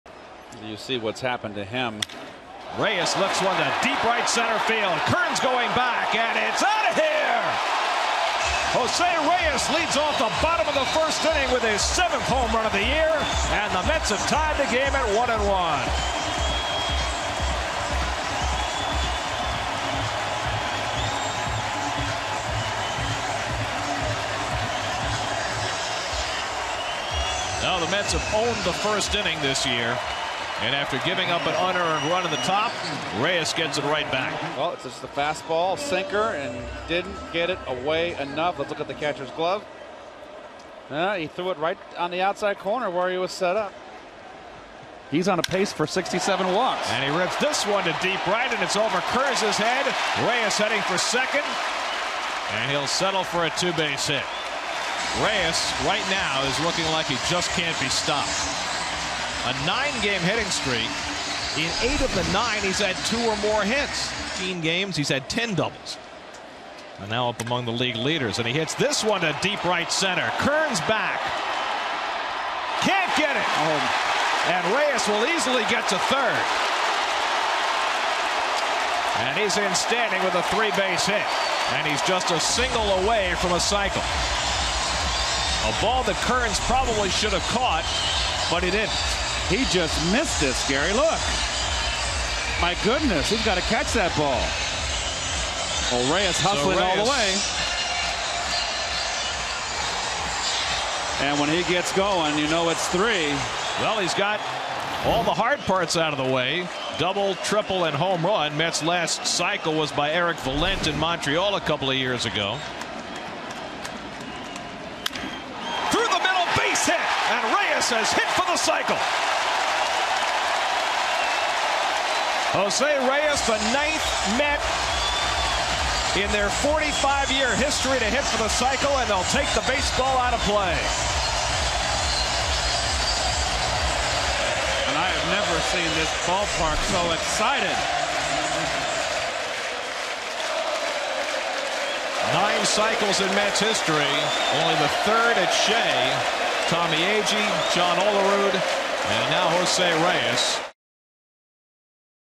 Reyes hits for the cycle at Shea Stadium in 2006
6/21/06: Jose Reyes hits for the ninth cycle in New York Mets history, going 4-for-5 in a loss to Cincinnati